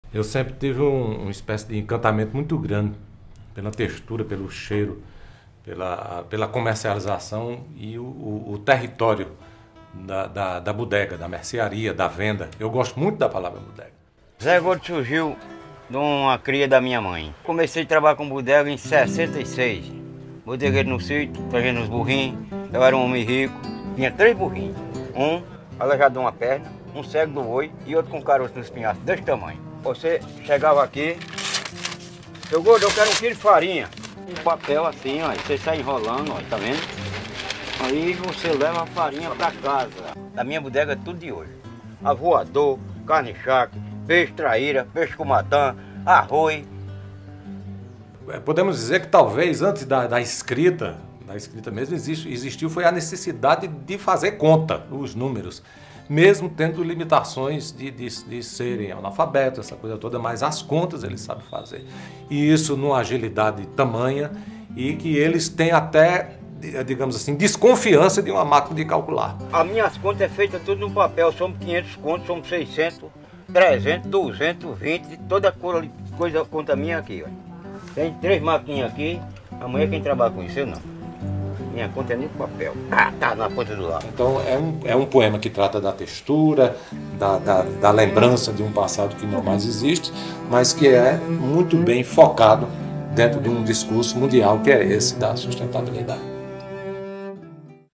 Papel de Bodega - Jessier Quirino
"Papel de bodega do pé da balança
De cores tão cinza, tão verde e encarnado
Que fica do lado do papel de seda
Que até a manteiga embrulhava em retalhos."

Trecho do poema Papel de Bodega de @jessierquirino

Esse como os outros filmes, também me alegra em ver que conseguimos extrair um pouco da essência da bodega do interior. Pra completar essa série que estão fazendo 10 anos nesse mês de junho, apresento-vos Papel de Bodega.

Agradeço mais uma vez a @jessierquirino e a @vitorquirino, grandes parceiros de longas datas. E claro que não vou deixar de agradecer a minha equipe que esteve junto para produzir esses documentários.
Ao diretor de fotografia @m.quixaba, que trouxe essa linguagem poética e maravilhosas, ao meu assistente de direção @jppalitot conduziu comigo todas as histórias, a @carine_fiuza que esteve a frente da produção, correndo atrás da logística, alimentação e tomando conta da gente e ao @brunodasilvassauro pela captação do som direto, sem vocês nada disso teria acontecido. Meu muito obrigado!

Equipe técnica:
Direção: @felipelimafilmmaker
Assist. Direção: @jppalitot
Dir. de Fotografia: @m.quixaba
Som direto: @brunodasilvassauro
Produção: @carine_fiuza e @vitorquirino
Gravado em Junho de 2013